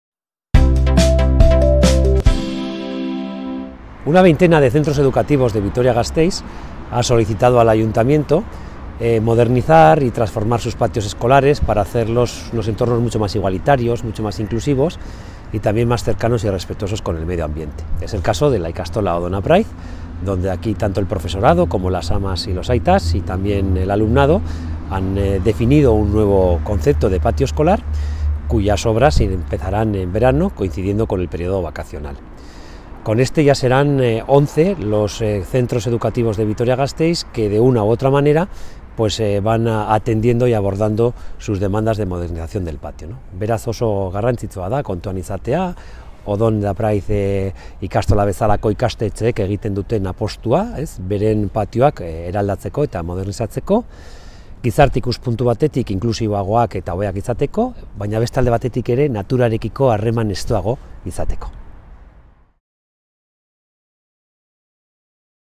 Alkatea Zurekin - Avanza la naturalización de patios escolares
Gasteizko 20 ikastetxek patioak modernizatzea eta eraldatzea eskatu diote udalari, iraunkorragoak izan daitezen.

Con Odón de Apraiz ya serán 11 los centros educativos de Vitoria-Gasteiz con sus patios naturalizados.